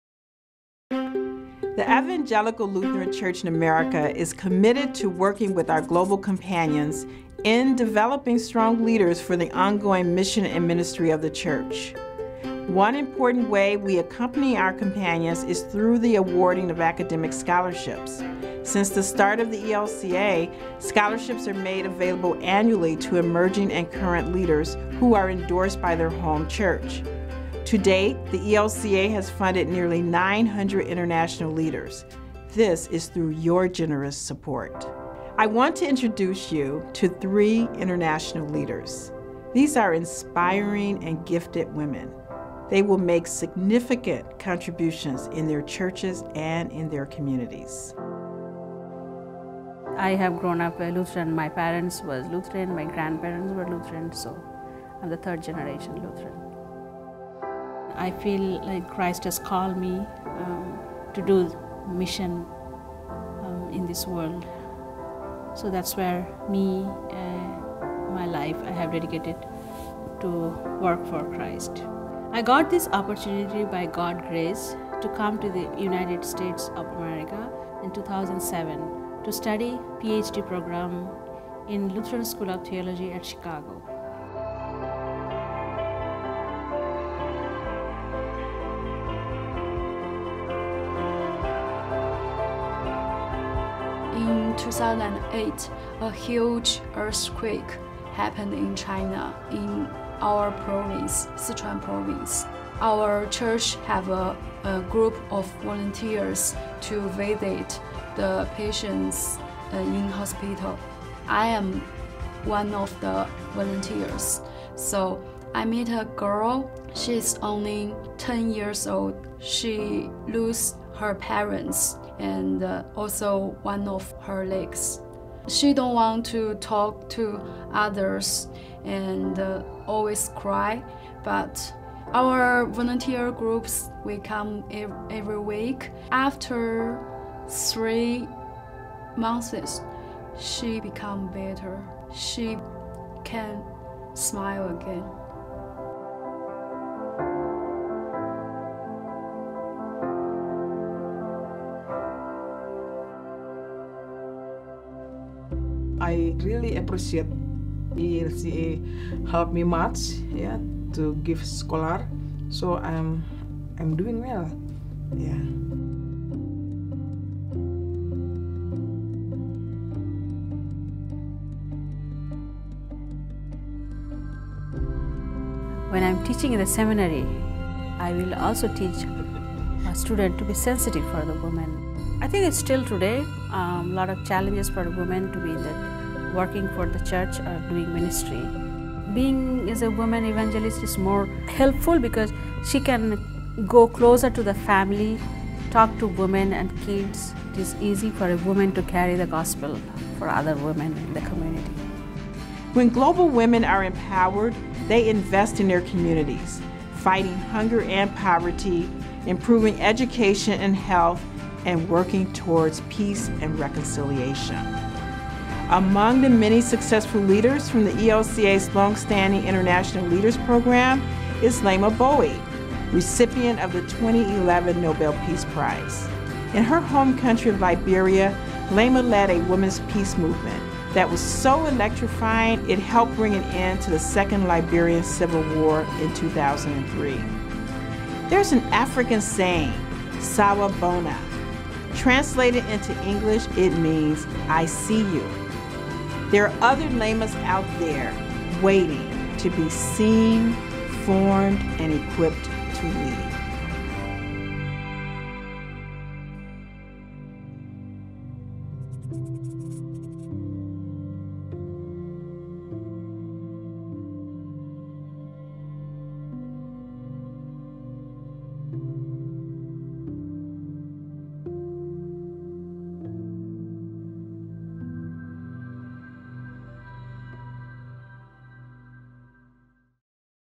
Meet 3 International Leaders
-- Meet Sanchita, Sitie and Anna, three women who’ve participated in the ELCA’s International Leaders program. International Leaders are members of our global companion churches who receive scholarships from the ELCA to advance their education so they can serve in faith-based leadership roles in their communities. Now, as part of Always Being New: The Campaign for the ELCA, we have an opportunity to raise up more women like them as leaders.